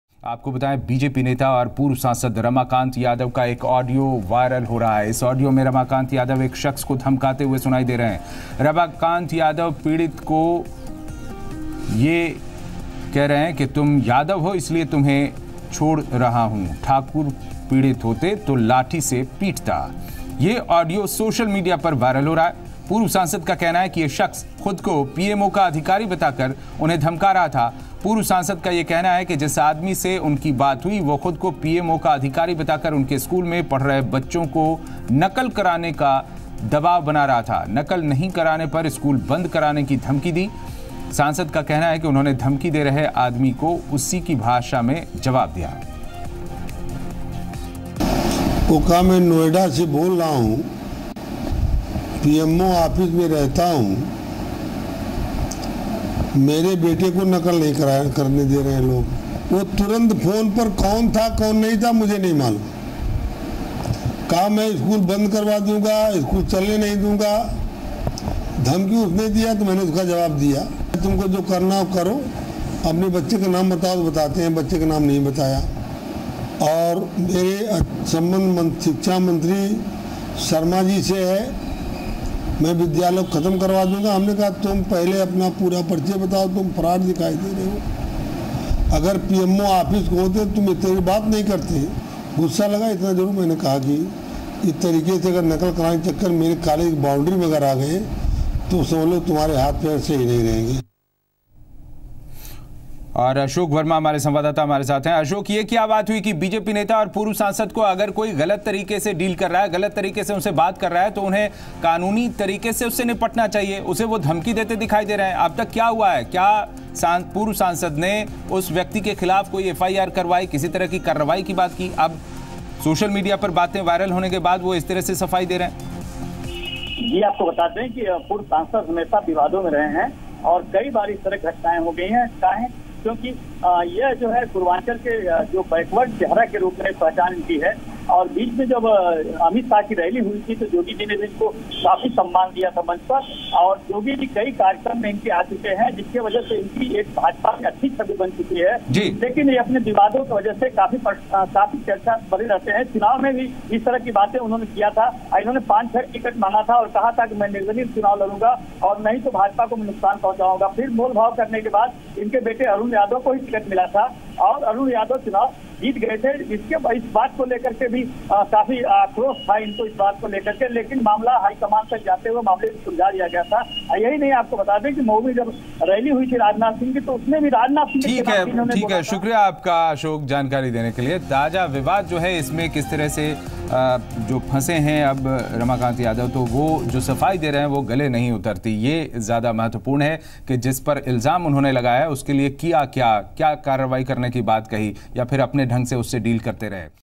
Audio of BJP leader Ramakant Yadav gone viral
Audio of BJP leader Ramakant Yadav gone viral. Watch this special segment and get to know more here.